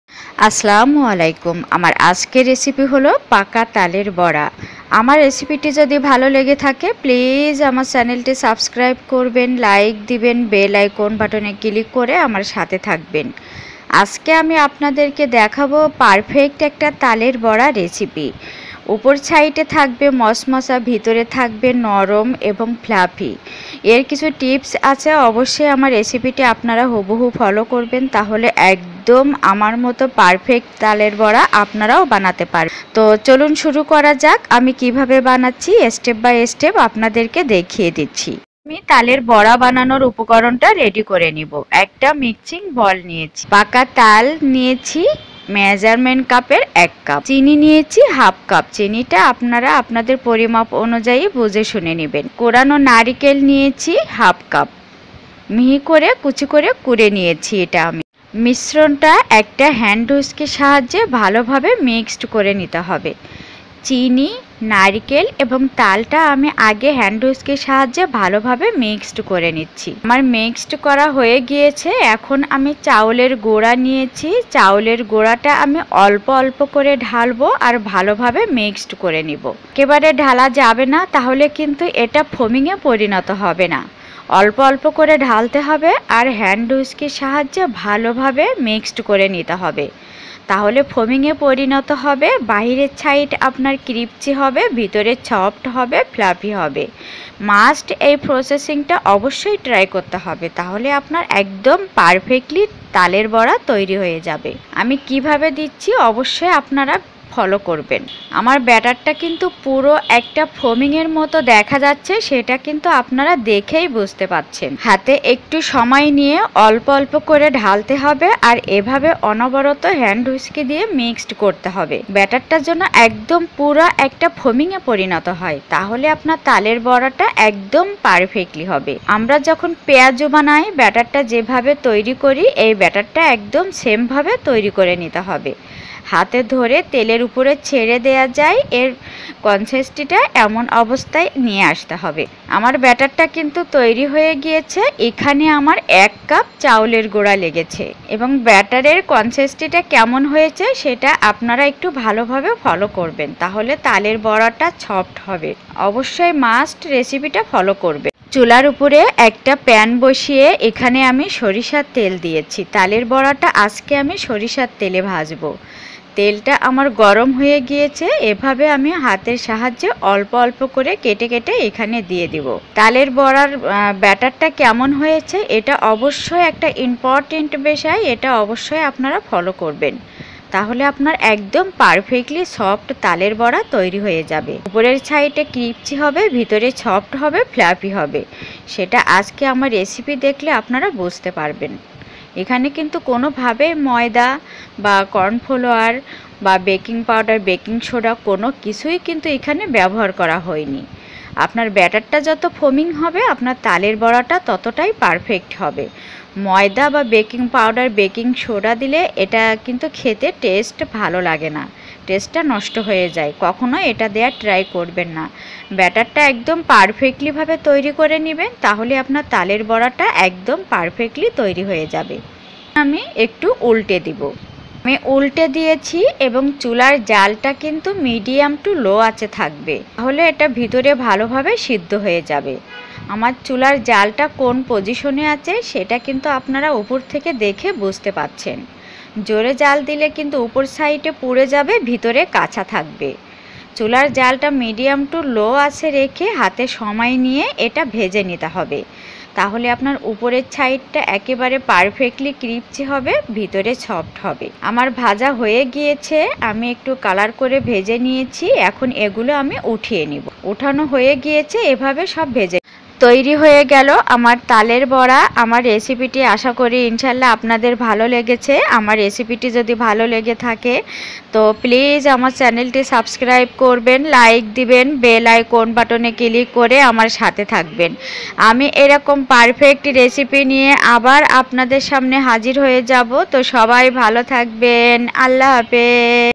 শুধুমাত্র চাউলের গুড়া, চিনি আর নারিকেল দিয়ে মুচমুচে তালের বড়া।Crispy Soft Fluffy Perfect Taler Bora।
বাহিরে মচমচা ভিতরে নরম ফ্লাফি তালের বড়া।Crispy Soft Fluffy Perfect Taler Bora।Appetizer।Taler Pitha।তালের বড়া এভাবে বানালে বাহিরে মচমচা ভিতরে নরম ফ্লাফি হবে।তার জন্য কিছু টিপস আছে।সেই টিপস গুলো ফলো করলে একদম পারফেক্ট হবে।আমার রেসিপি যদি ভালো লেগে থাকে প্লিজ আমার চ্যানেলটি সাবস্ক্রাইব করবেন লাইক দিবেন বেল আই কোন বাটনে ক্লিক করে আমার সাথে থাকবেন।